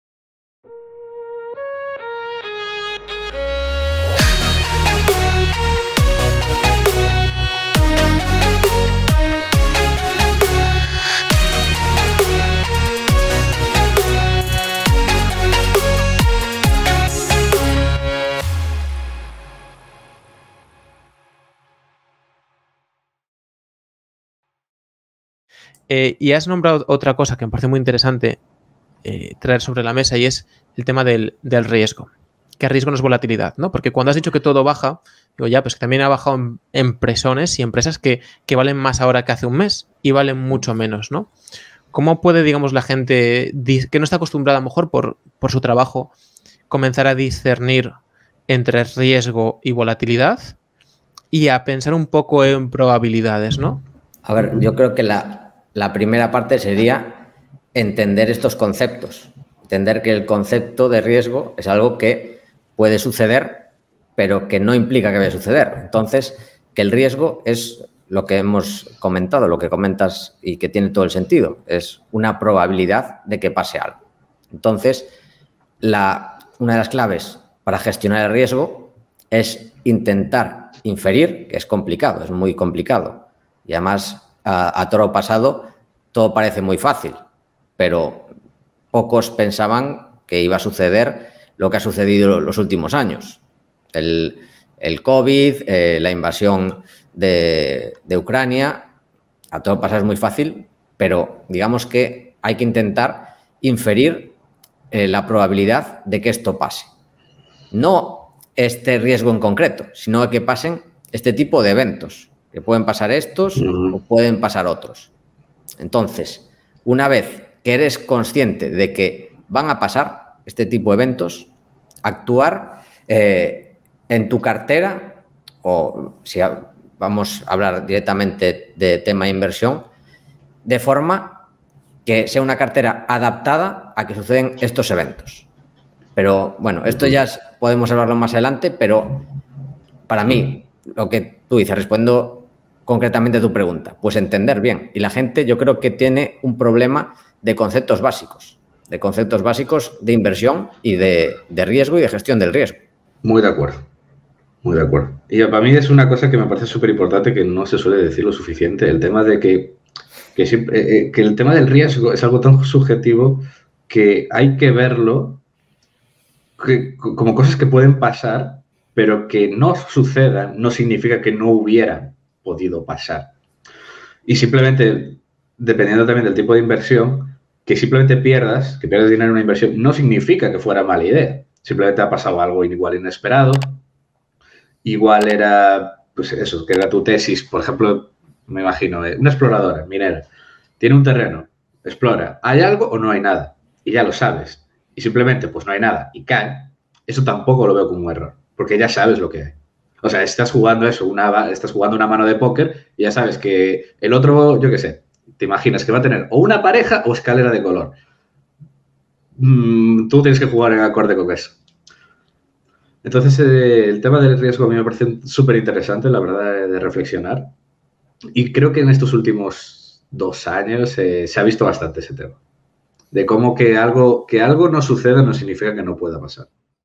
¿Riesgo o Volatilidad? 🤔 con Adrián Godás & Paco Lodeiro
Adrián Godas y Paco Lodeiro tienen una larga trayectoria en el mundo de la inversión siendo especialmente conocidos en el panorama nacional español.
Ambos son anfitriones de uno de los Podcast de inversión con mayor repercusión en el sector de la inversión en español, Value Investing FM.
Además ambos llevan analizando empresas e invirtiendo muchos años.
También tienen formaciones y comunidades digitales para todos aquellos que también son unos apasionados por el mundo de la inversión.

Esto es un fragmento de la conversación en el Podcast #63 de Falla con Éxito.

TIMESTAMPS

0:53 - Presentación
1:43 - Cambios recientes en como invertir
4:30 - Diferencias entre riesgo y volatilidad
11:06 - Geopolítica como factor de Riesgo
18:30 - ¿Porque te fuiste de España?
21:08 - Que es y en que te afecta la Inflación
27:31 - Como prevenir consecuencias de la inflación
37:28 - Dudas de alumnos de finanzas en época de crisis
42:31 - ¿El gobierno puede regular tus inversiones?
49:48 - ¿Qué es La Tasa Tobin?
51:18 - Consecuencias financieras del conflicto Rusia Ucrania
1:05:29 - Preguntas breves finales
1:17:54 - Pregunta a David Huerga.